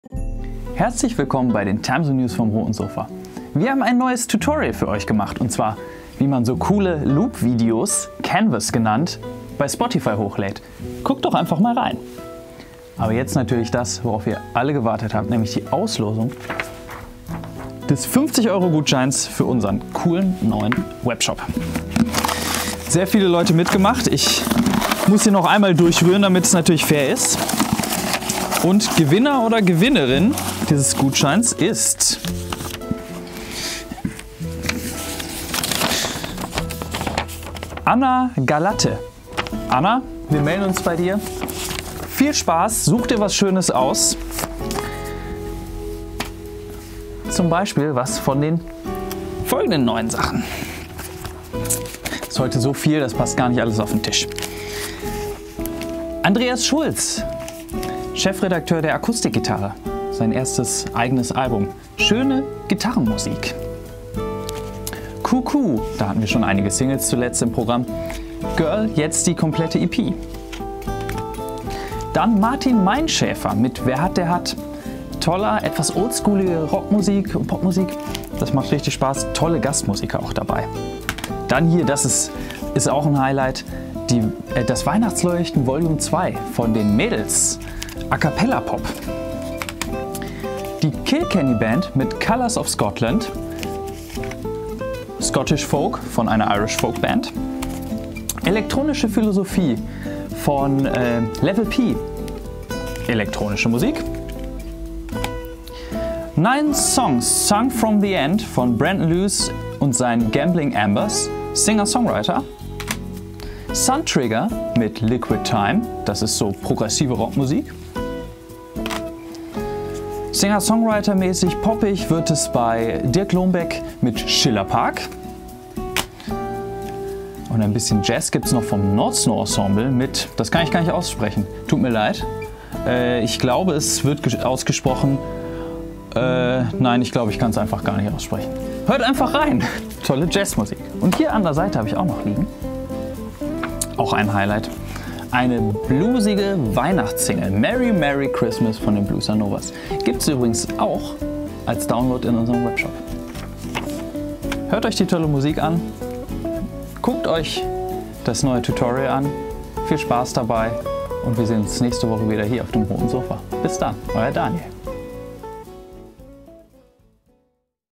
Verlosung des Gutscheins: Timezone News vom 27. November 2020
+++ 🍄 Herzlichen Glückwunsch 🍀 +++ … an die Gewinnerin des EUR 50,– - Gutscheins vom Timezone-Webshop. Folgende Acts haben heute Neuheiten herausgebracht: Nordsnø Ensemble, Level π, Dirk Loombeck, Suntrigger, Andreas Schulz, Kilkenny Band, Brendan Lewes And The Gambling Ambers, Medlz, Martin Meinschäfer, The Bluesanovas, CouCou. Viel Spaß beim Hören! ❤️